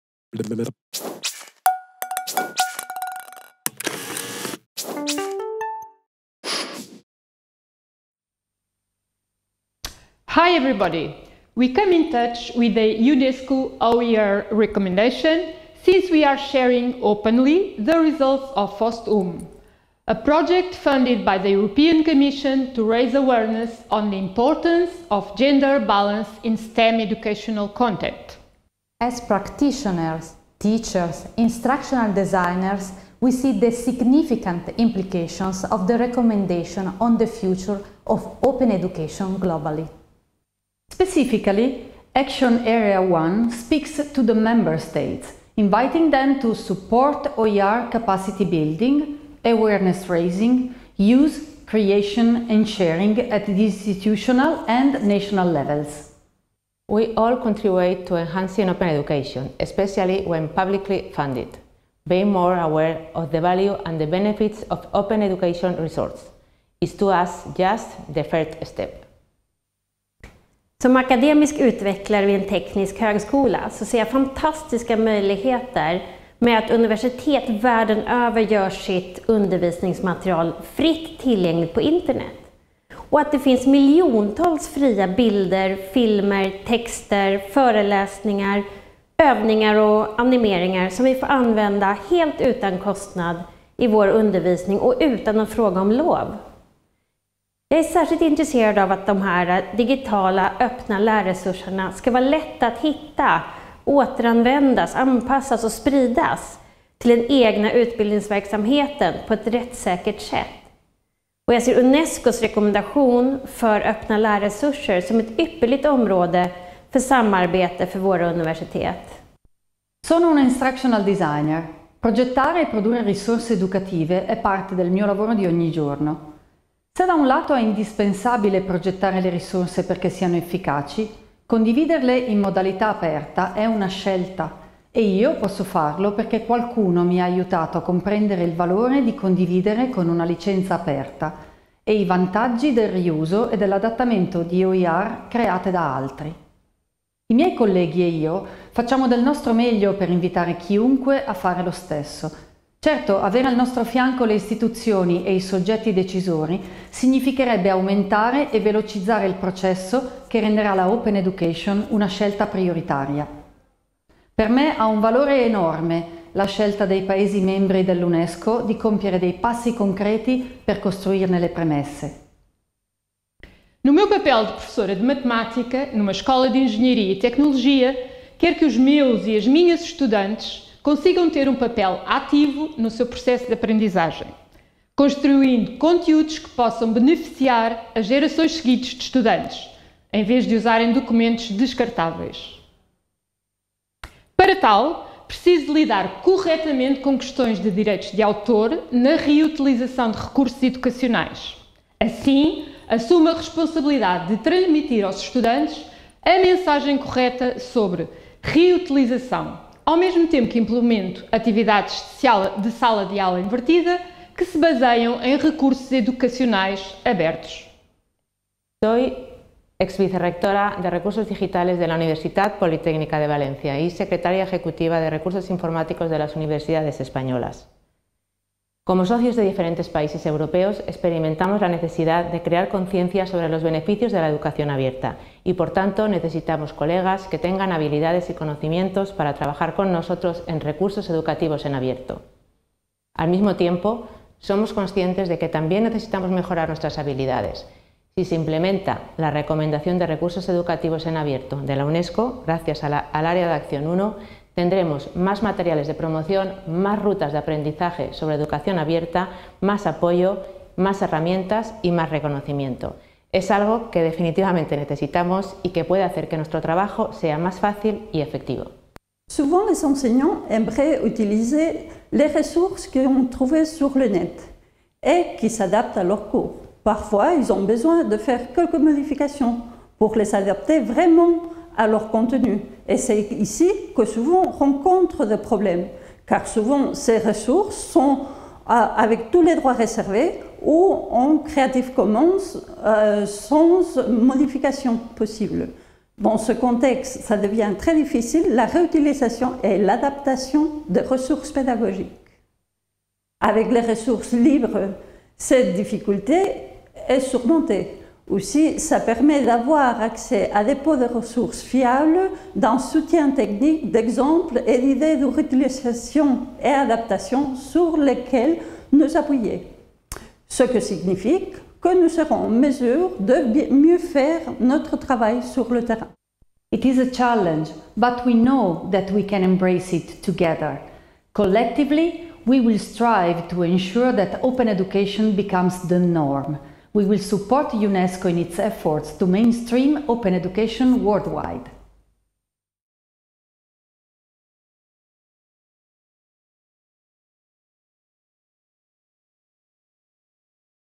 Open Educators reflect on the UNESCO OER Action Area: Building Capacity
This introduction to the first of the UNESCO OER Recommendation action areas is from members of the FOSTWOM project. Together they explain what the action area is about in English, and then each adds, in their own language – with English subtitles – what this means in their work as a teacher, an instructional designer, a gender balance expert, and a community manager.

The members are:
* Ana Moura Santos, faculty member of the Department of Mathematics Técnico Lisboa, University of Lisbon, Portugal
* Valieria Baudo, Project Manager at MeTID, Politécnico de Milano, Italy
* Paola Corti, Project Manager at METID, Politécnico de Milano, Italy
* Virginia Vega Carrero, Professor at the Faculty of Business Administration and Management of the UPV, Spain
* Charlotta Nilsson, Teacher, KTH, Sweden
* Astrid Rosso, Igénieure pédagogique et Conservatoire national des arts et métiers, Paris, France

Timestamps:
0:10 – English : Ana Moura Santos, Portugal
0:28 – English : Valeria Baudo, Italy
0:40 – English :  Paola Corti,  Italy
0:56 – English :  Virginia Vega Carrero, Spain
1:10 – Swedish : Charlotta Nilsson, Sweden
1:59 – Italian : Paola Corti, Italy
2:52 – Portuguese : Ana Moura Santos, Portugal
3:37 – Spanish : Virginia Vega Carrero, Spain
4:27 – French : Astrid Rosso, France
5:25 – English : Paola Corti concludes with a collective call to action in English